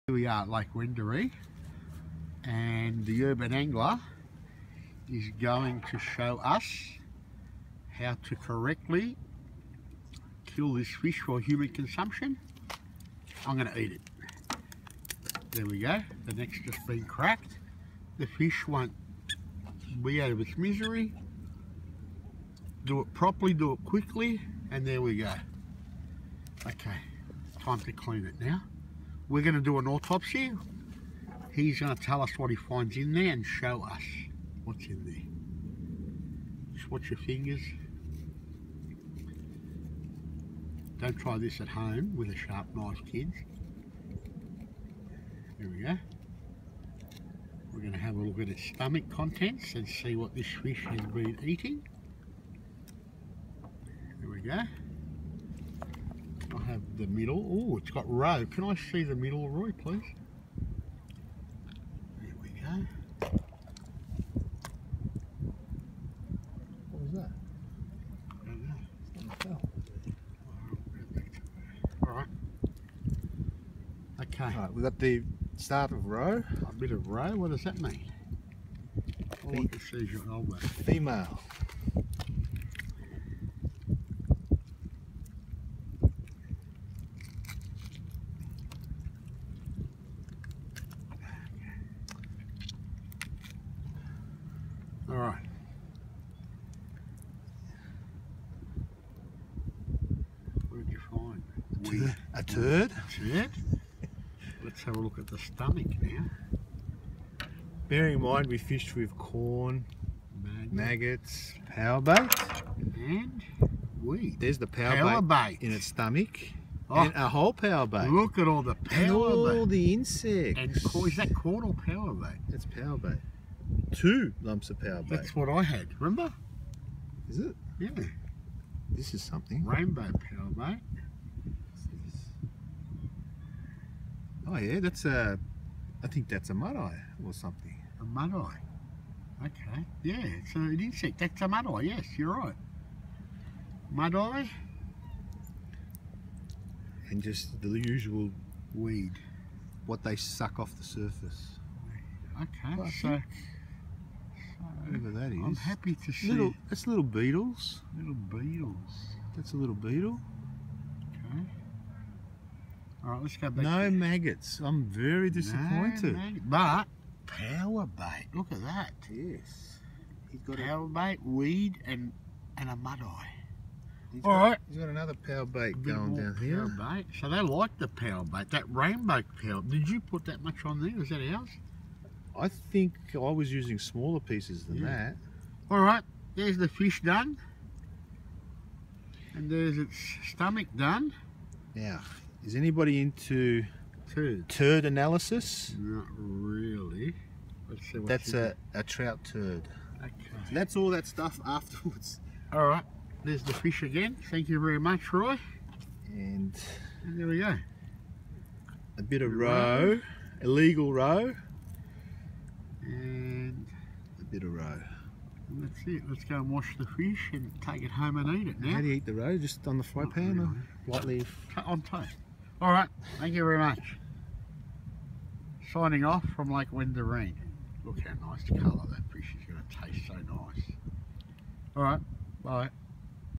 Autopsy a Brown trout at lake Wendouree to see what they are feeding on
John Regali Autopsy of a Brown trout at lake Wendouree to see what the trout are feeding on